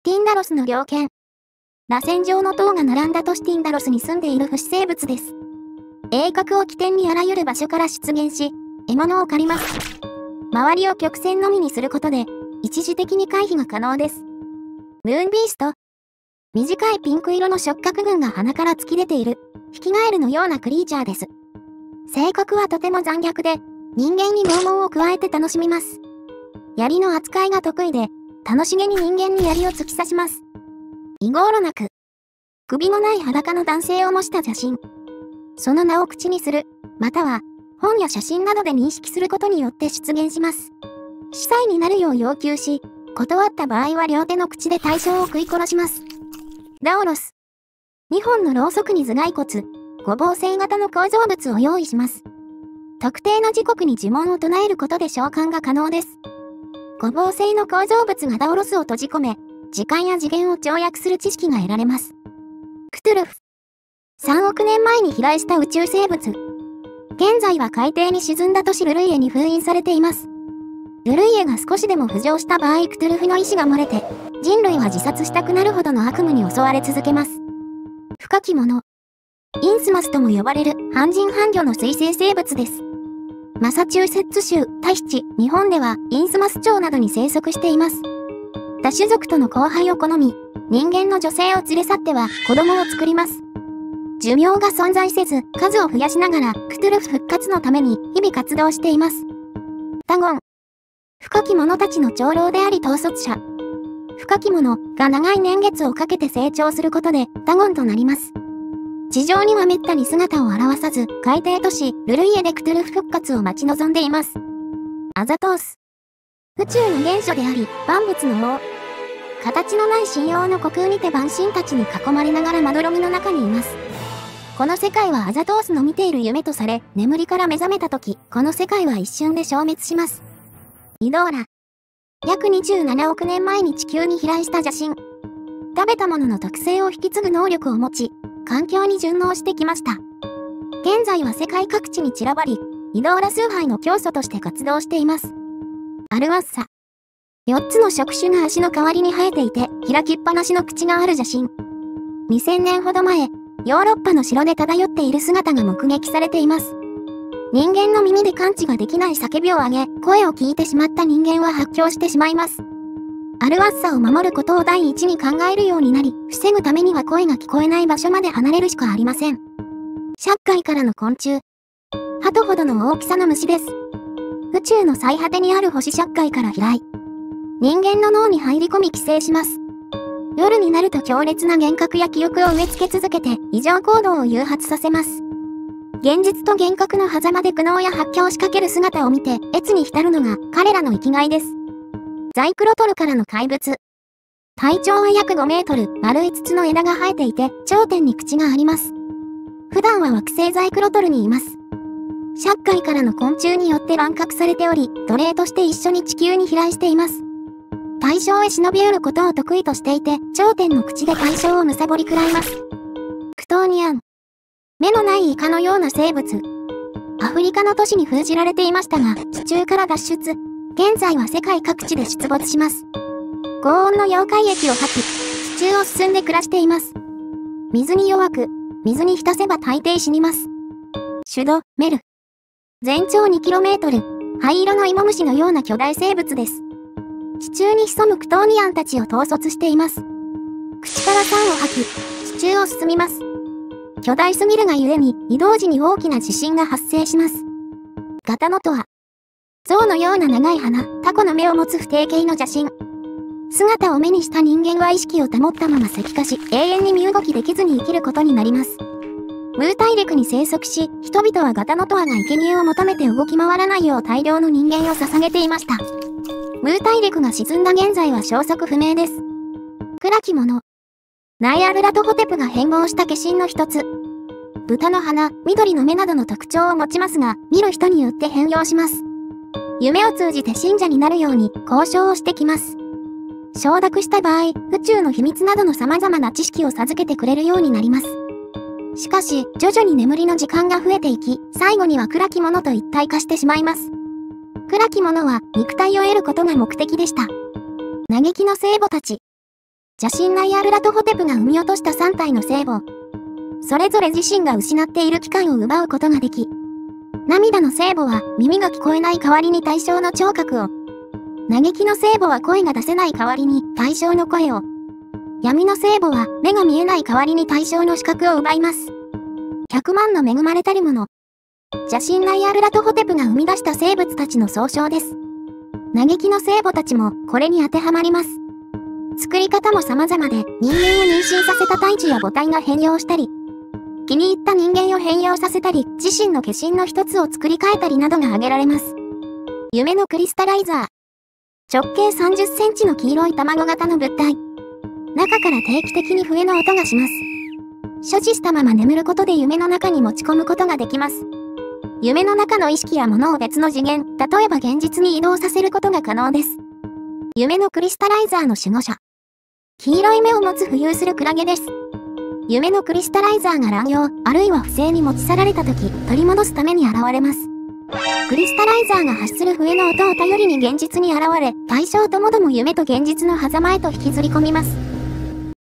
総集編：クトゥルフ神話③
ショート動画の総集編③です

00:00 ティンダロスの猟犬
00:15 ムーンビースト
00:30 イゴーナロク
00:47 ダオロス
01:02 クトゥルフ
01:18 深きもの
01:40 ダゴン
01:55 アザトース
02:10 イドーラ
02:26 アルワッサ
02:50 シャッガイからの昆虫
03:12 ザイクロトルからの怪物
03:33 クトーニアン
03:51 シュド=メル
04:08 ガタノトア
04:30 暗きもの
04:58 嘆きの聖母たち
05:19 百万の恵まれたるもの
05:39 夢のクリスタライザー
05:58 夢のクリスタライザーの守護者

音源：効果音ラボ / OtoLogic / On-Jin ～音人～